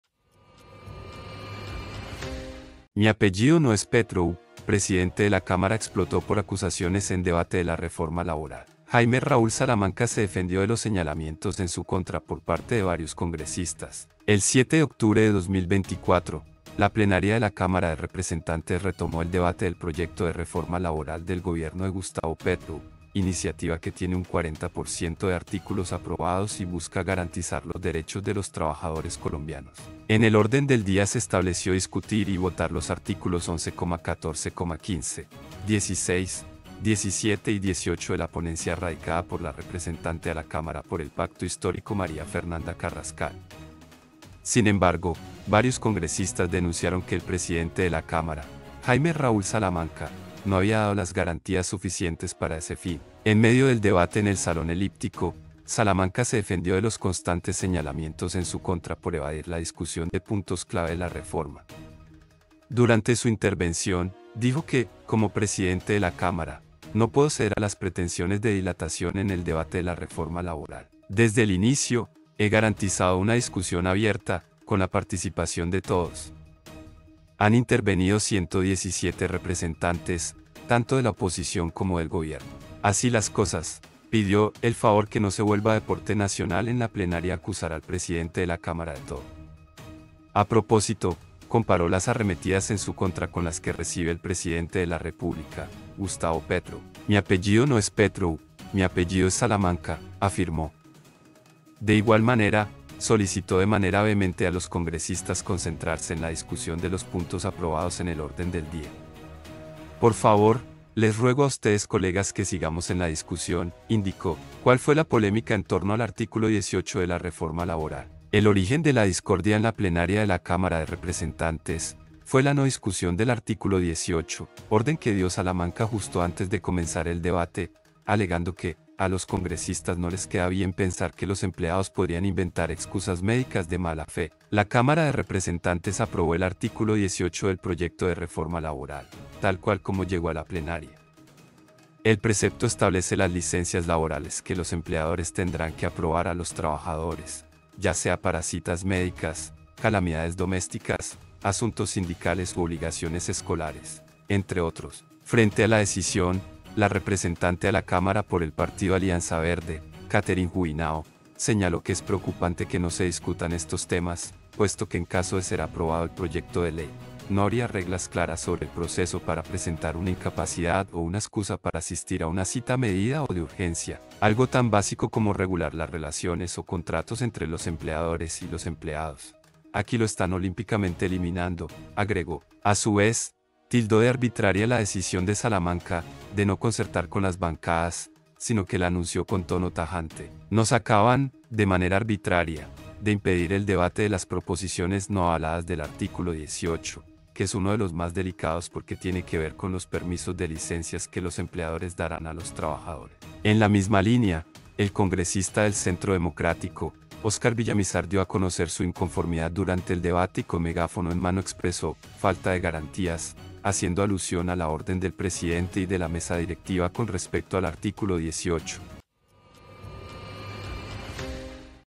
URGENTE 🔵 PRESIDENTE DE LA CÁMARA EXPLOTÓ POR ACUSACIONES EN DEBATE DE LA REFORMA LABORAL
“Mi apellido no es Petro”: presidente de la Cámara explotó por acusaciones en debate de la reforma laboral
Jaime Raúl Salamanca se defendió de los señalamientos en su contra por parte de varios congresistas
INFOBAE